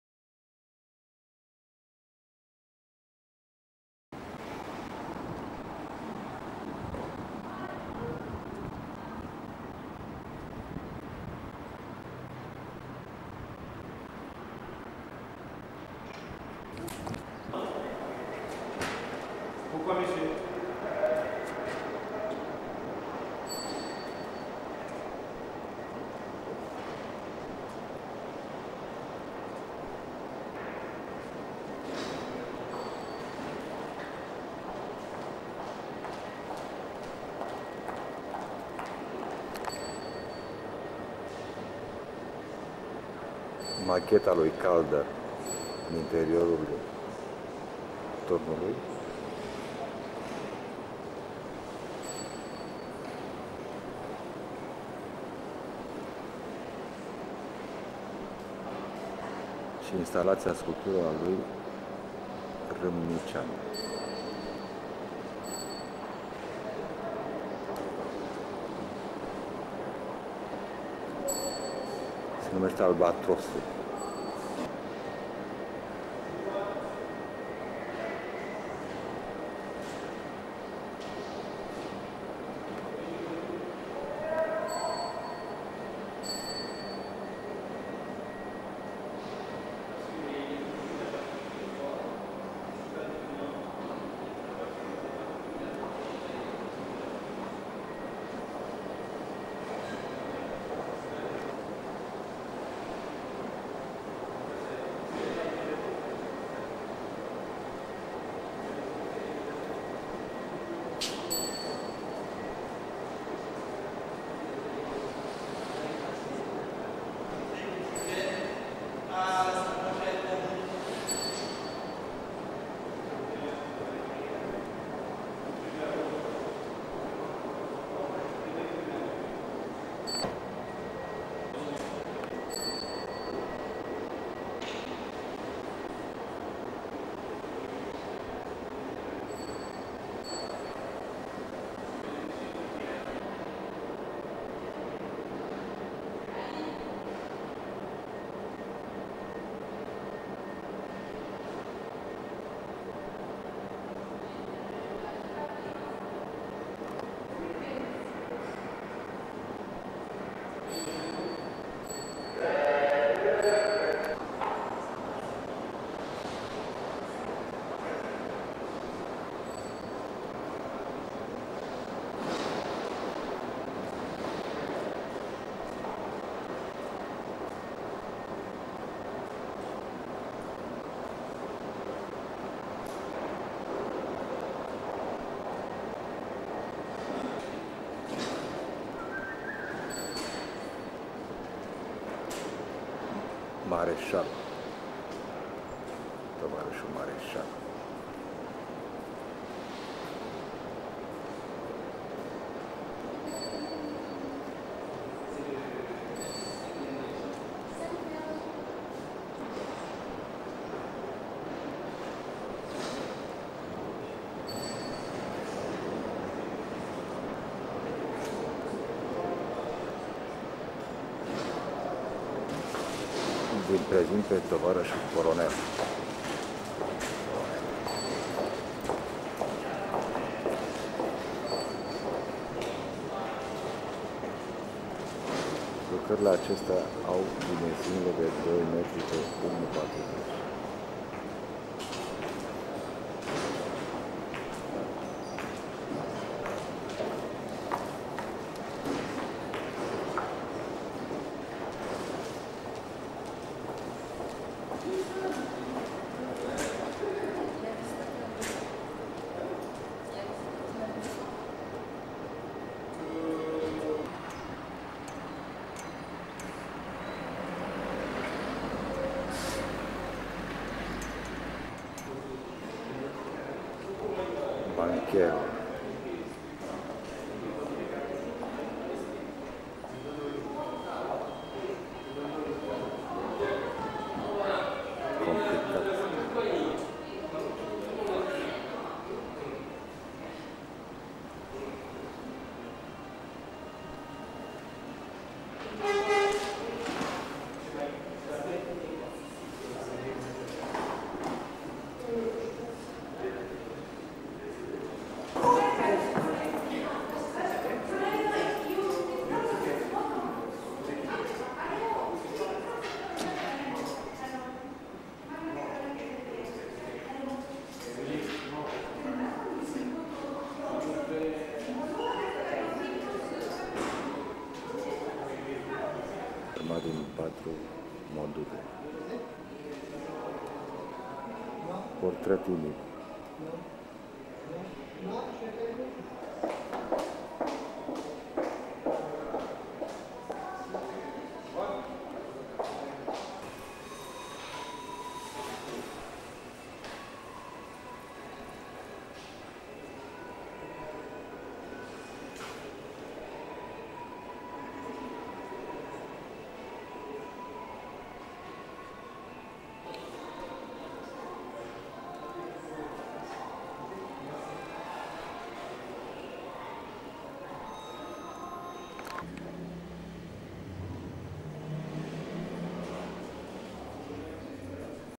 Stefan Ramniceanu's art exhibition at La Défense, Paris
A behind-the-scenes look at French-Romanian contemporary artist Stefan Ramniceanu's 2003 exhibition at the La Défense - Areva, Sofitel.
Reportage sur l'exposition réalisée en 2003 par l'artiste franco-roumain Stefan Ramniceanu à la Défense dans la tour Areva et à l'hôtel Sofitel.
Expoziția din 1992 lui Stefan Ramiceanu la Défense in Paris, in turnul Areva.